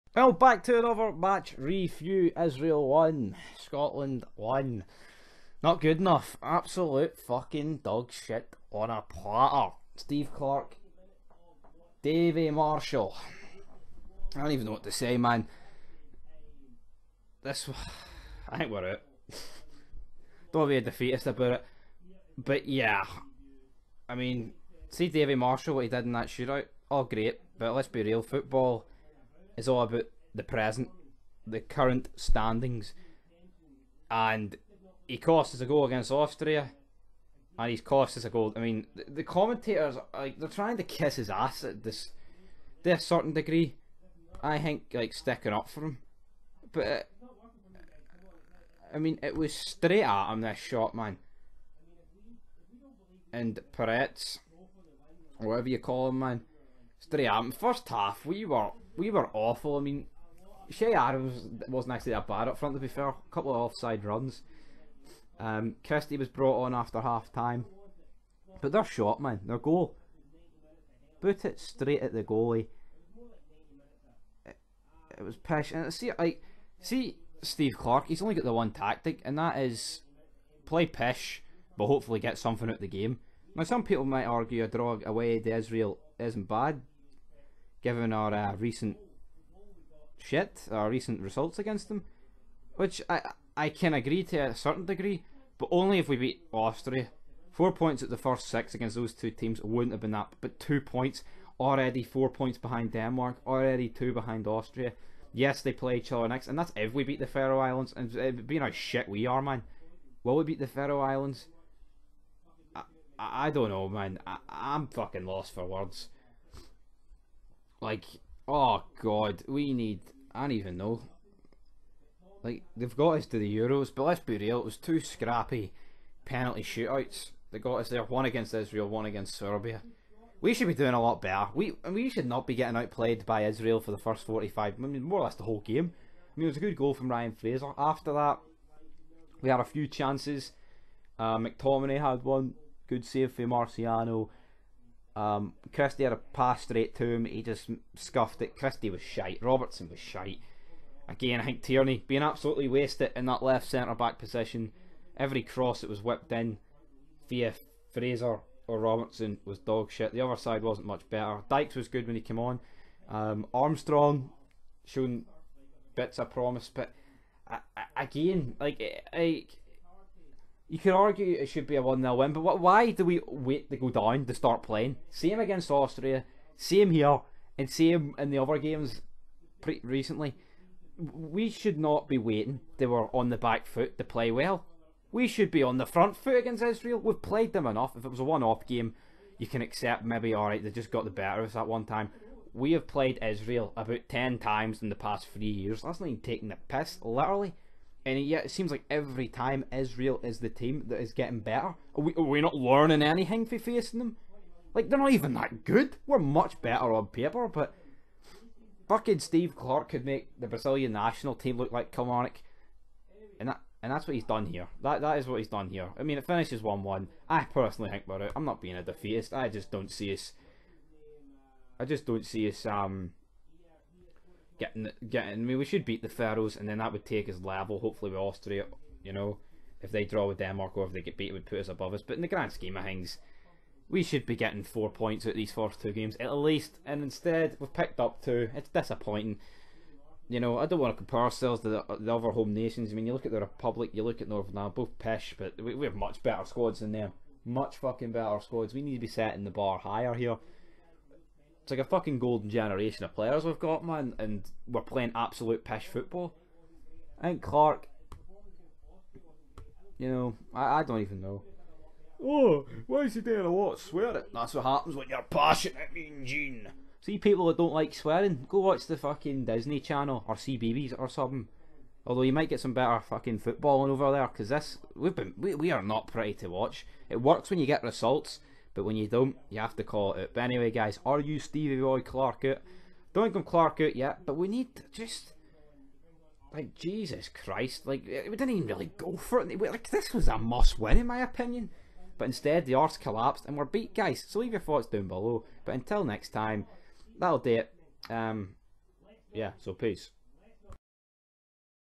NOT GOOD ENOUGH! ISRAEL 1-1 SCOTLAND - MATCH REVIEW - WORLD CUP 2022 QUALIFIER
WE ARE OUT! NAE CHANCE